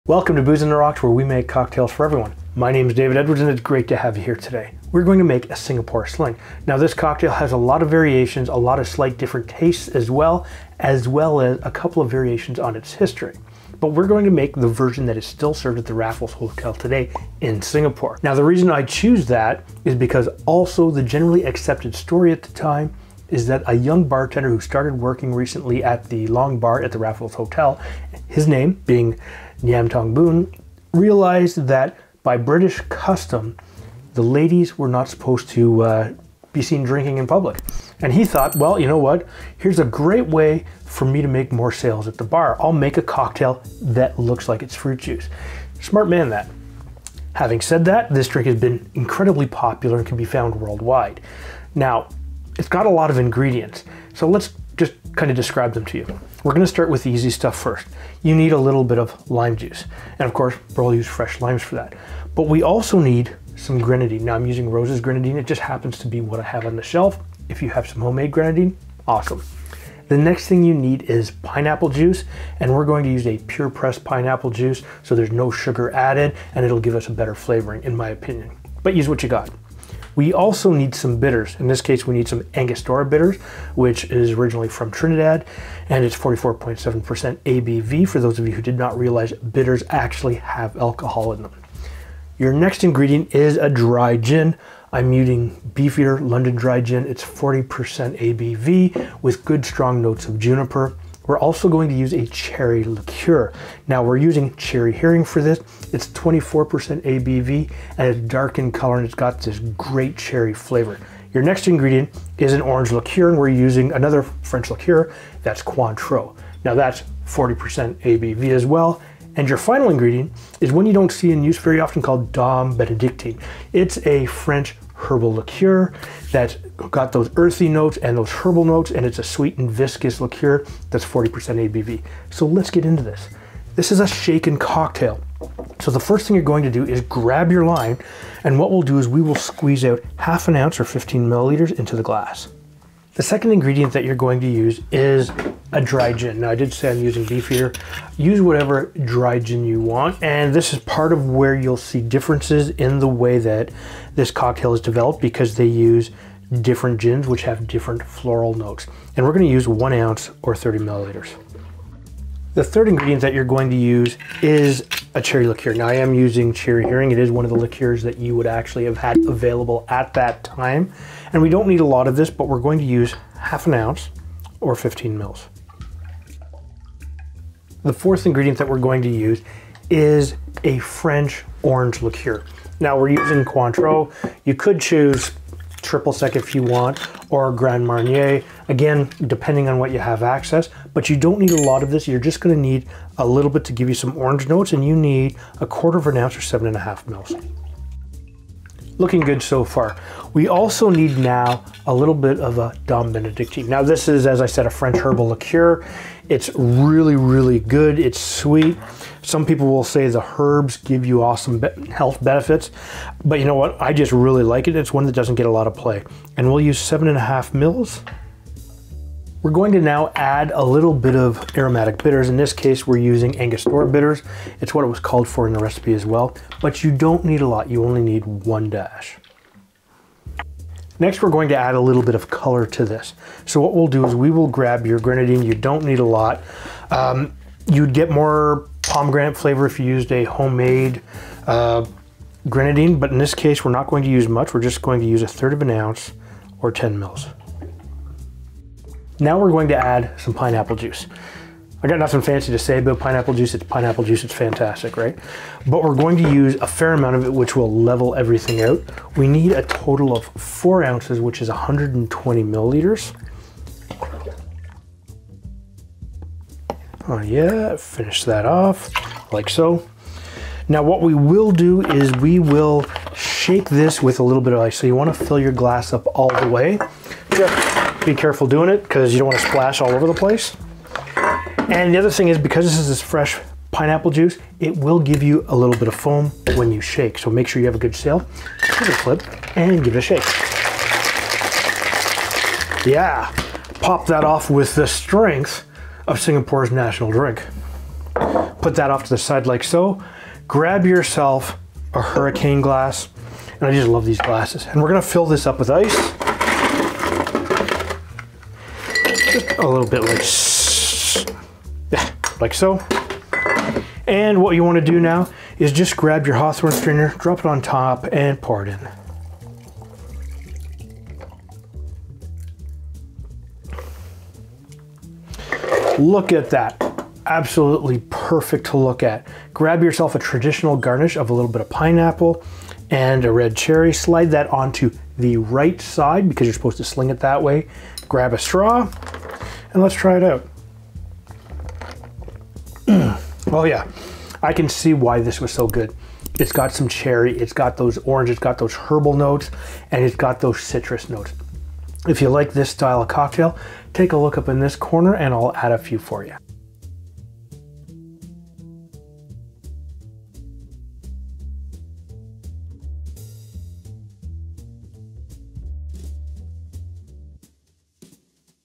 How To Make The Singapore Sling
Today on Booze On The Rocks, we make the Singapore Sling cocktail served at the Raffles Hotel. Created by Chinese-born Ngiam Tong Boon in 1915 at the Long Bar in the Raffles Hotel, the Singapore Sling has passed the test of time. Although there are now, many versions found all over the world. Gin, Grand Marnier, cherry liqueur, herbal liqueur (typically Benedictine), pineapple, lime, bitters, and club soda are the main ingredients in this complex drink. The recipe was first made public in Harry Craddock's masterpiece "The Savoy Cocktail Book," released in 1930. It became the subject of subsequent recipes, and by 1948, David A. Embury claimed in his book "The Fine Art of Mixing Drinks" that no two published recipes were identical. The Singapore Sling is sophisticated, subtle, and ingredient-heavy at its best. It also begs for investigation, and s s a result, broad notes and nostalgia serve as the foundation for most "traditional" cocktail variations. Many contemporary variations tend to be excessively sugary beverages that depend more on grenadine and pineapple juice than on herbal liqueurs and fresh citrus. This recipe is as authentic to the original as one can make it. Try it out and let me know what you think.

BAR TOOLS

SINGAPORE SLING

Dry Gin 1 oz/30 ml
Cherry Liqueur .5 oz/15 ml
Orange Liqueur .25 oz/7.5 ml
DOM Benedictine .25 oz/7.5 ml
Angostura Bitters 1 Dash
4 oz/120 ml Pineapple Juice
.5 oz/15 ml Lime Juice
.3 oz/10 ml Grenadine

Method

1. Add all of the ingredients to a shaker tin.
2. Shake with ice for 10-15 seconds.
3. Fine strain into a Hurricane glass over fresh ice.
4. Garnish with a pineapple chunk and cocktail cherry perched on the righht side of the glass.